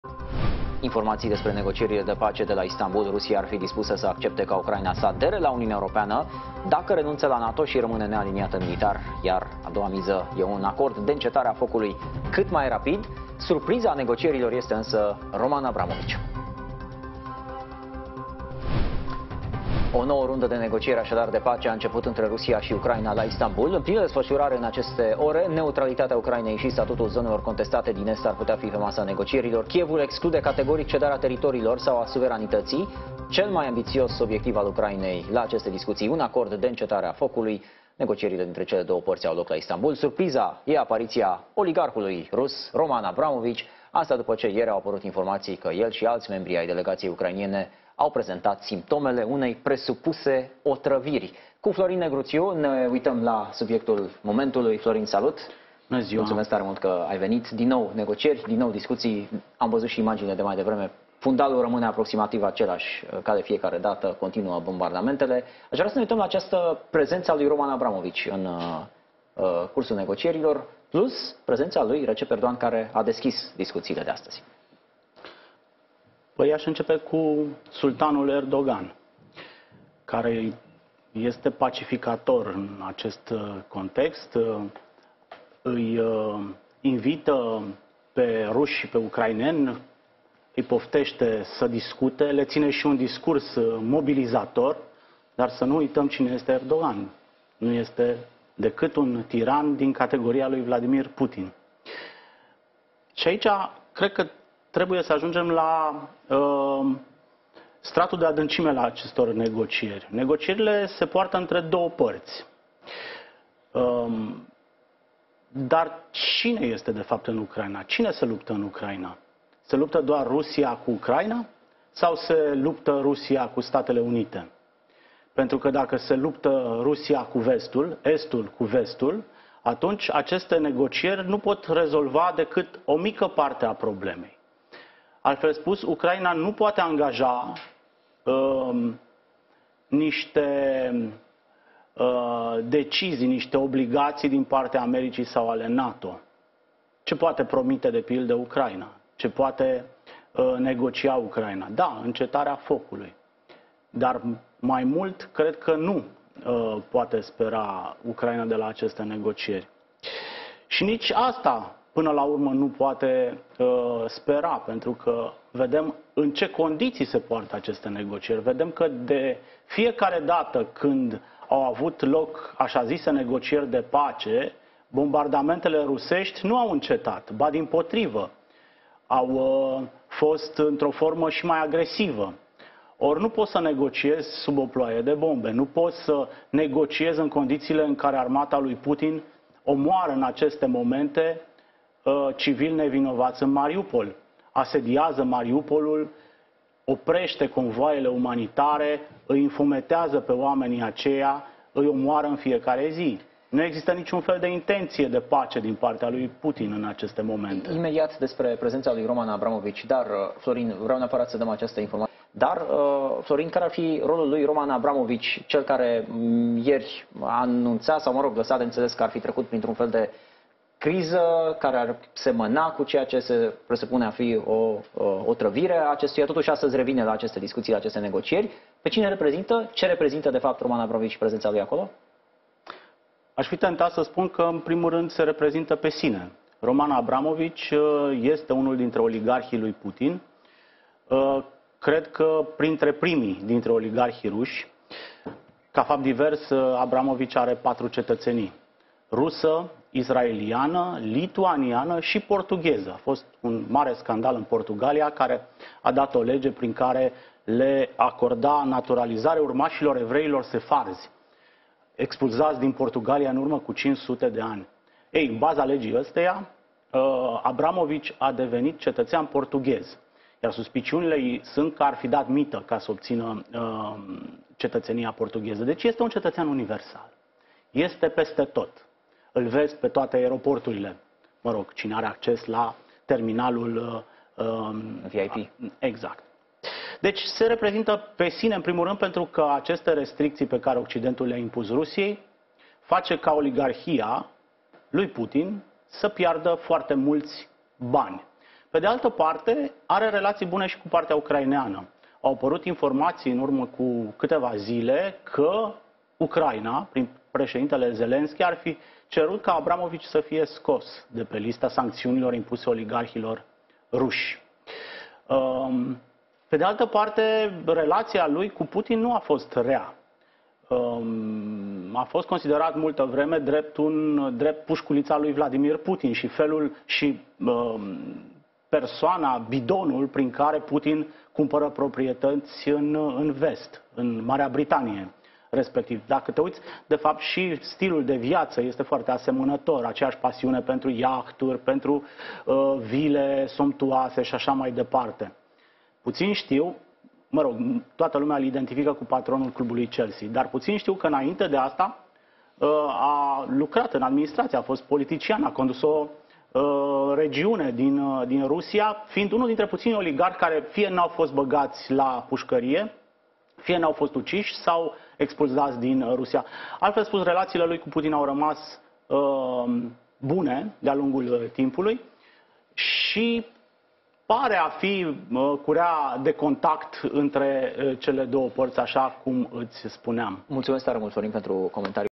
Florin Negruțiu: Erdogan nu este decât un tiran din categoria lui Vladimir Putin
Negocierile dintre Ucraina şi Rusia au început în Turcia marţi, fără o strângere de mână, transmite televiziunea ucraineană, informează Reuters. „A fost o primire rece, fără strângeri de mână", a relatat un reporter ucrainean, fără a preciza dacă el a fost martorul acestei noi întrevederi între delegaţii sau a fost informat din surse oficiale.

➥ Război în Ucraina. Delegaţiile ucraineană și rusă se întâlnesc la Istanbul